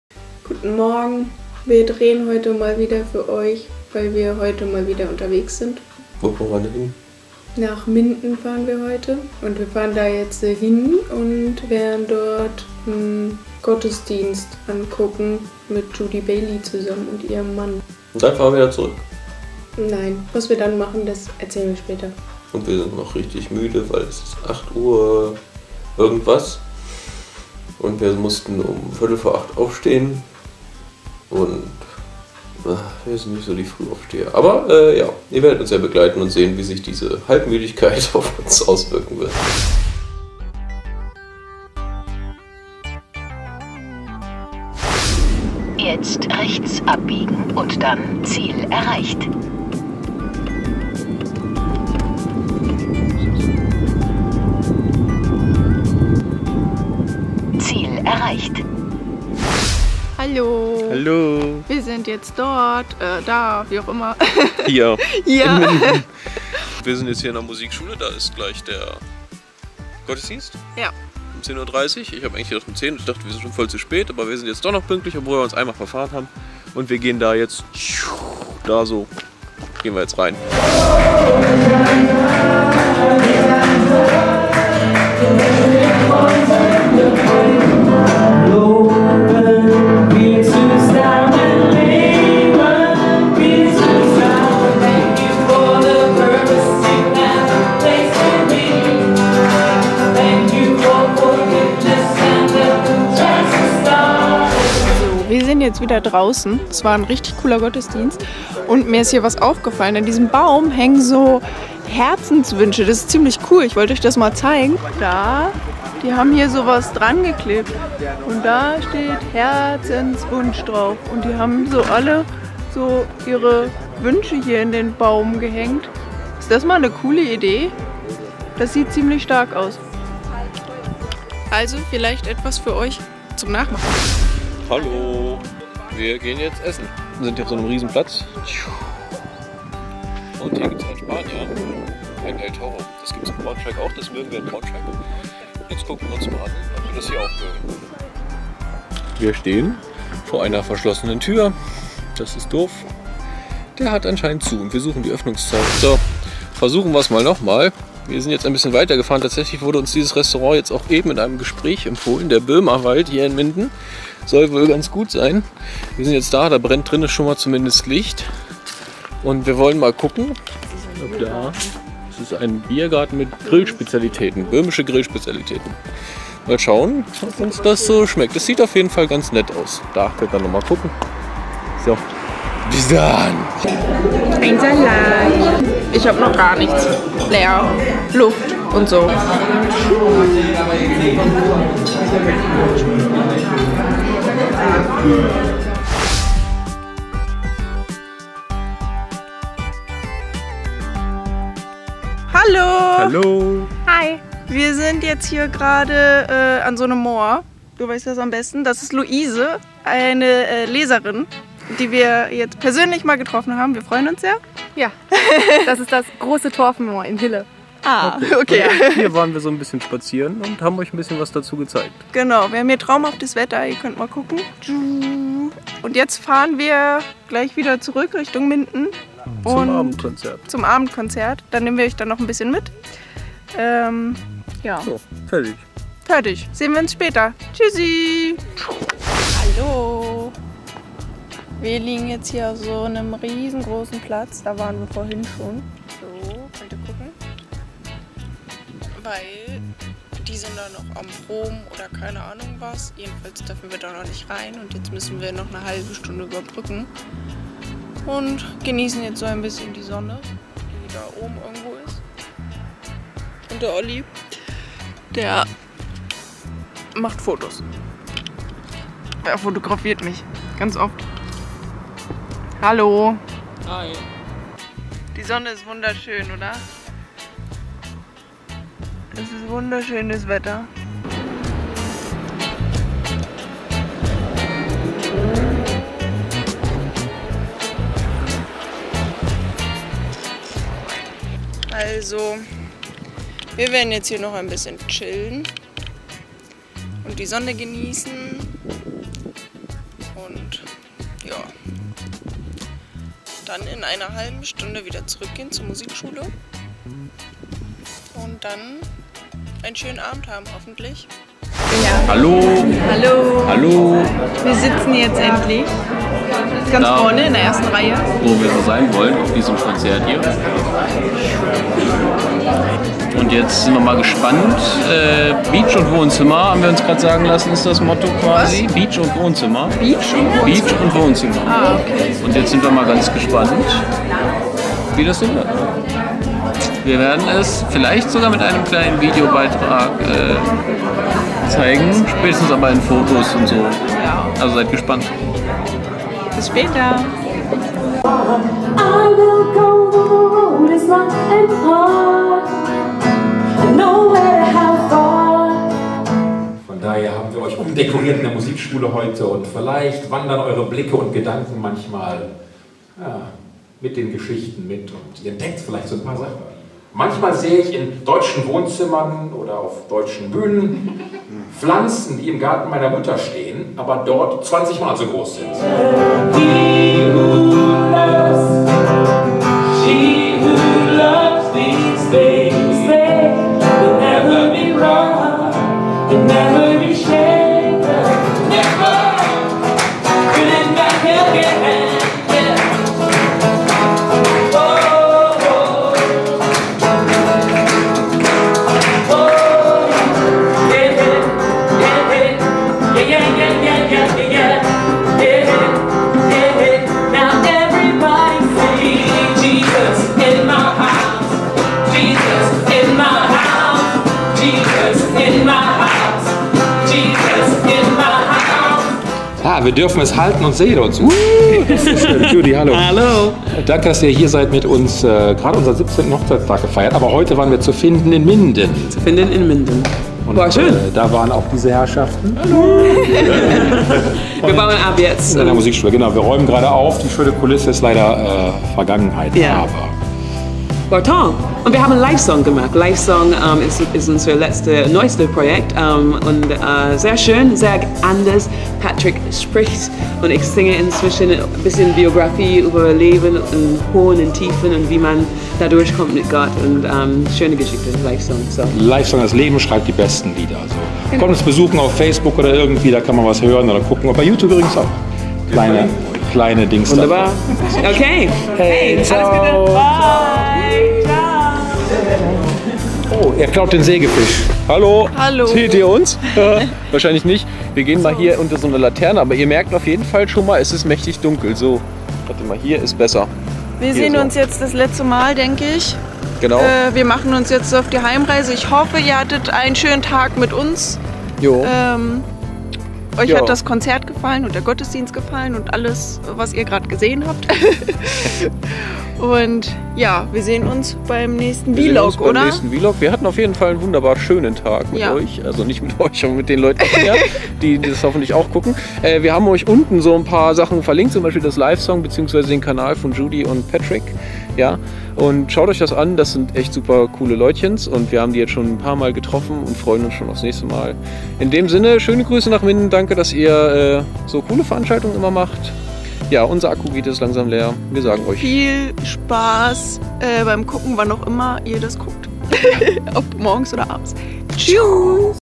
Judy Bailey und Patrick Depuhl - Lifesong - VLOG
Judy Bailey und Patrick Depuhl, "zu finden in Minden".

Lifesong - mit ihrem neuen Konzert- und Lese-Abend sind beide derzeit on Tour.